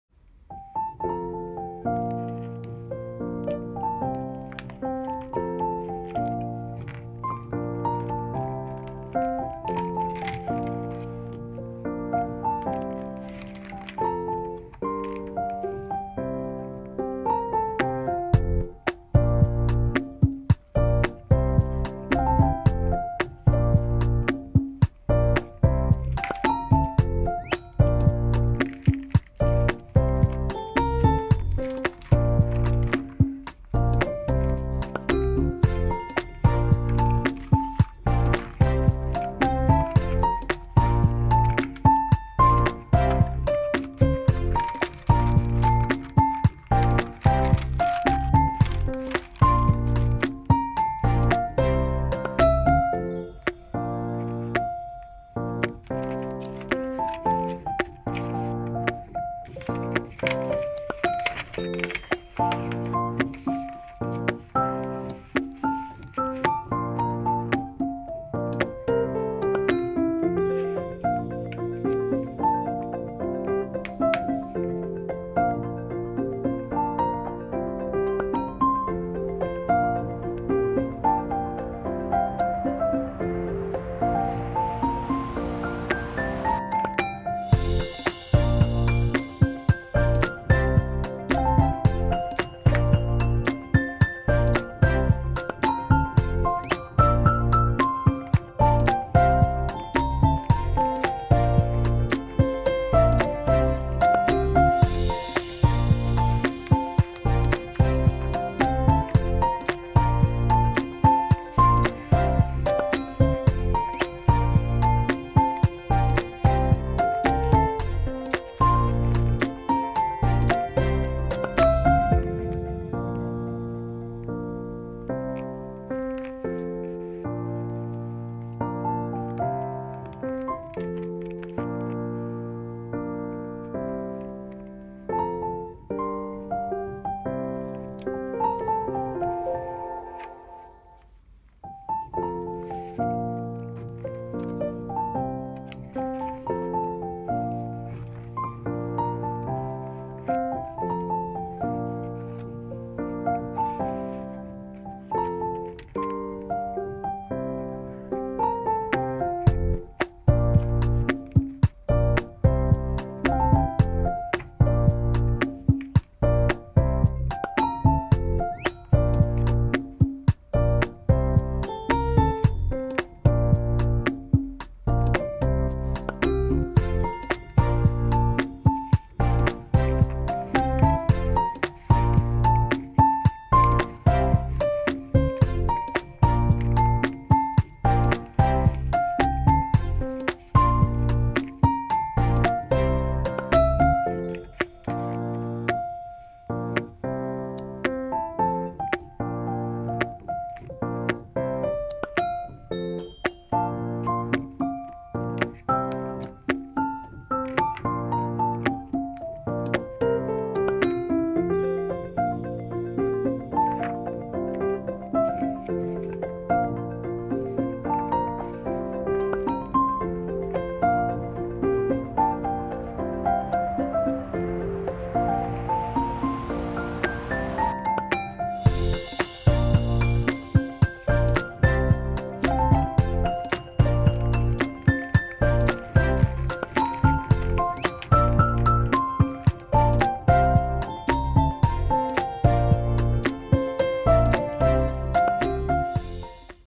Osaka Travel Journal #14 : final flip through | Journal With Me | 오사카 여행 다이어리 꾸미기 완성본
My Osaka travel journal is finally done !! I am sharing a flip through in this video. And if you are interested, there are a few videos also showing the processes in my channel.

오사카 여행 다이어리를 드디어 완성했습니다~!!
2박 3일 여행인데 다이어리는 2달쯤 걸린건가요ㅋㅋㅋ 더디지만 완성해서 너무 기분이 좋네요~ 들러주셔서 감사합니다!!